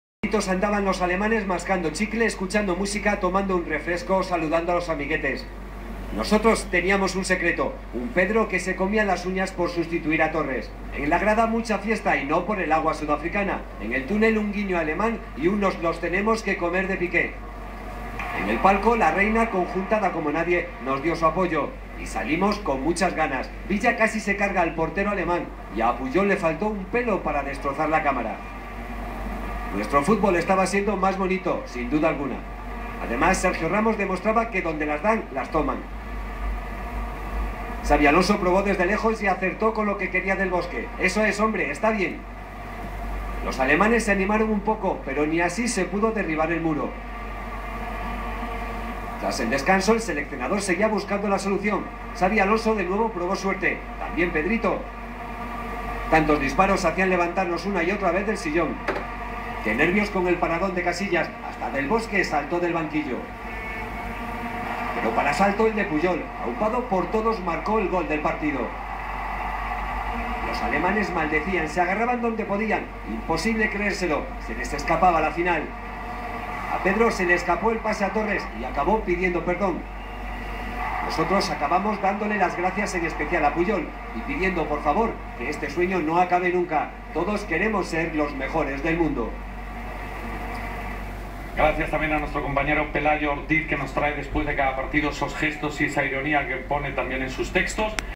resumen españa 1-0 alemania 2010
españa a la final de la copa del mundo 2010